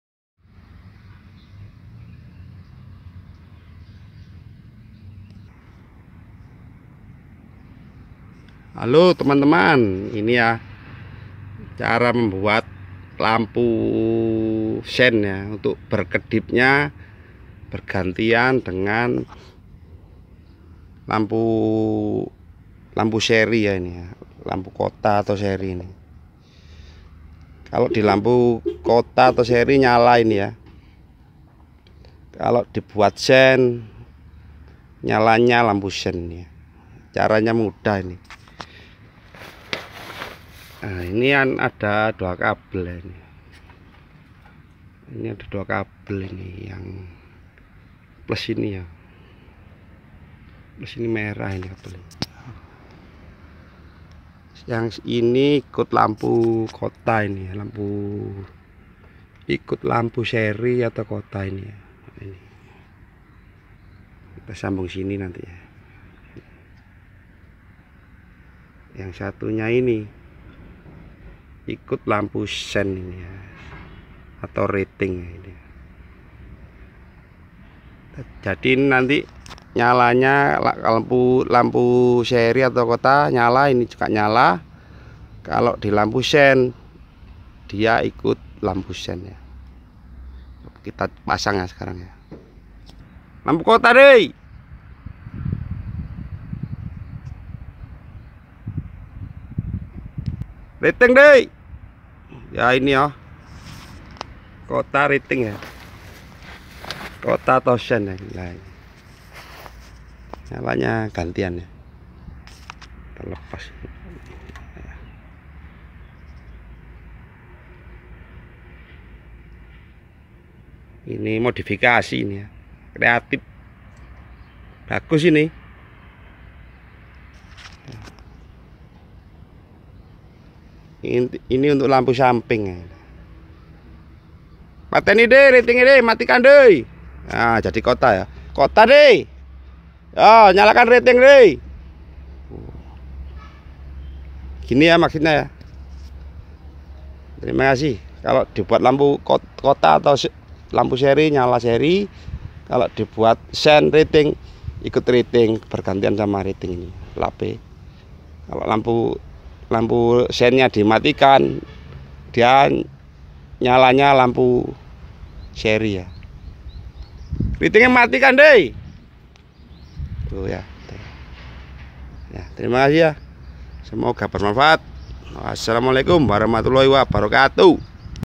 Membuat variasi sen berkedip bergantian dengan lampu kota
Cara Membuat lampu sen berkedip bergantian dengan lampu kota